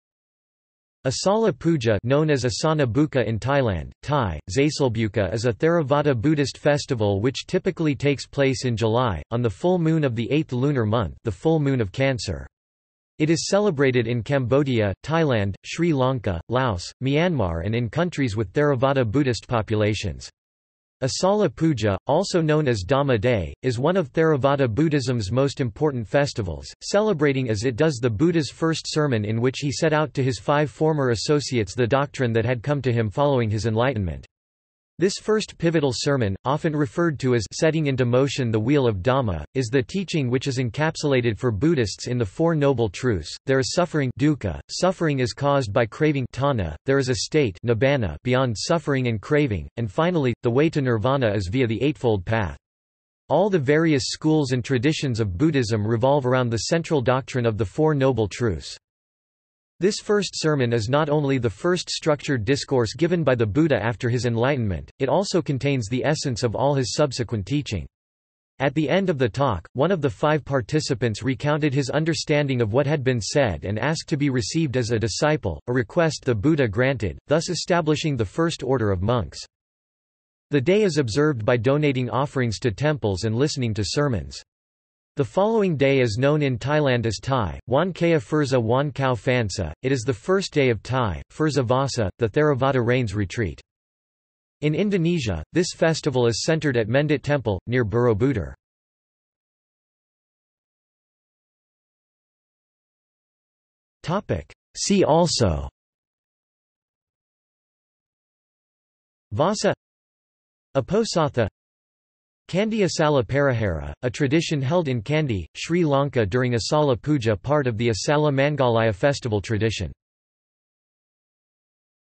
Asalha Puja | Wikipedia audio article
This is an audio version of the Wikipedia Article:
Asalha Puja

00:01:40 undefined

- Socrates

SUMMARY
Asalha Puja (known as Asanha Bucha in Thailand, Thai: อาสาฬหบูชา) is a Theravada Buddhist festival which typically takes place in July, on the full moon of the eighth lunar month (the full moon of Cancer). It is celebrated in Cambodia, Thailand, Sri Lanka, Laos, Myanmar and in countries with Theravada Buddhist populations. Asalha Puja, also known as Dhamma Day, is one of Theravada Buddhism's most important festivals, celebrating as it does the Buddha's first sermon in which he set out to his five former associates the doctrine that had come to him following his enlightenment. This first pivotal sermon, often referred to as “setting into motion the wheel of dhamma,” is the teaching which is encapsulated for Buddhists in the four noble truths: there is suffering (dukkha); suffering is caused by craving (tanha); there is a state (nibbana) beyond suffering and craving; and finally, the way to nirvana is via the eightfold path.  All the various schools and traditions of Buddhism revolve around the central doctrine of the four noble truths.
This first sermon is not only the first structured discourse given by the Buddha after his enlightenment, it also contains the essence of all his subsequent teaching. At the end of the talk, one of the five participants recounted his understanding of what had been said and asked to be received as a disciple, a request the Buddha granted, thus establishing the first order of monks.
The day is observed by donating offerings to temples and listening to sermons.  The following day is known in Thailand as Thai: วันเข้าพรรษา Wan Khao Phansa; it is the first day of Thai: พรรษา vassa, the Theravada rains retreat.
In Indonesia, this festival is centered at Mendut Temple, near Borobudur.